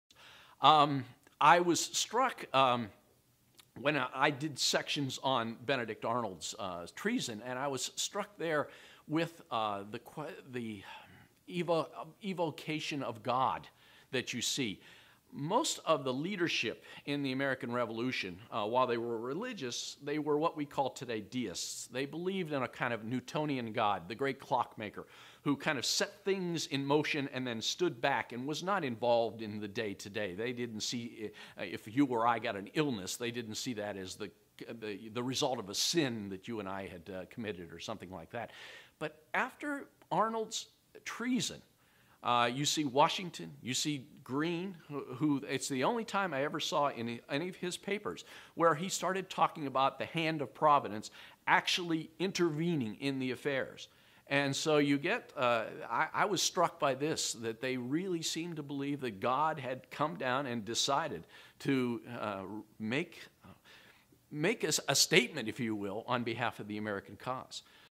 The Evocation of God During the American Revolutionary War
- Historian Dennis M. Conrad explains what he found interesting about the newspaper coverage of Benedict Arnold. For more information about Reporting the Revolutionary War, please